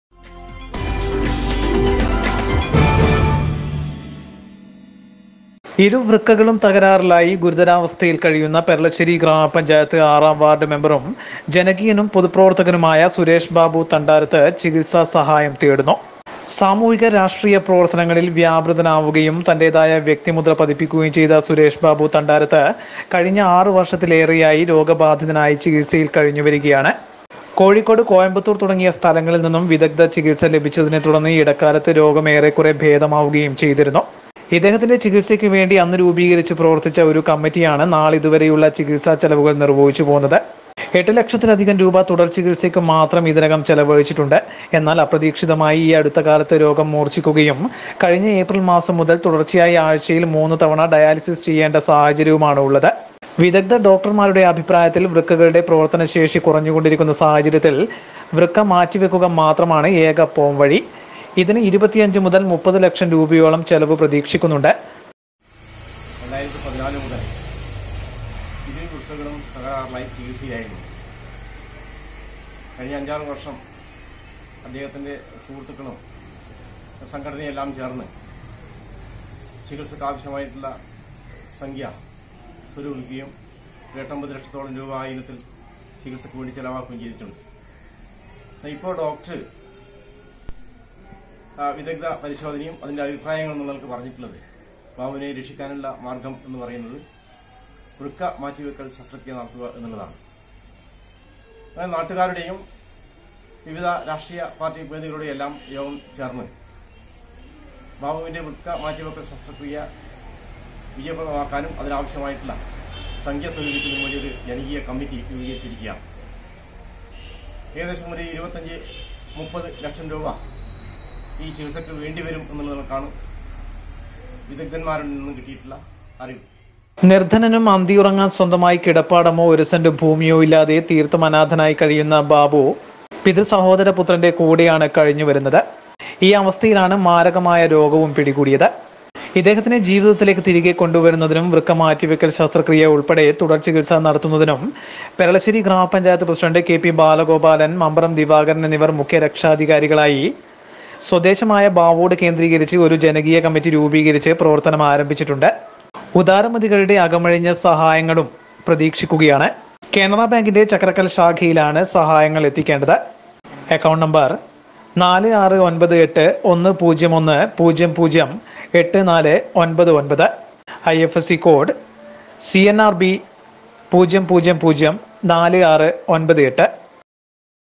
വൃക്ക മാറ്റിവെക്കാൻ സുമനസ്സുകളുടെ സഹായം തേടി സുരേഷ് ബാബു തണ്ടാരത്ത്.
കാരുണ്യത്തിന്റെ സ്നേഹസ്പർശത്തിന് . സുമനസ്സുകളെ ഒരു സഹായം...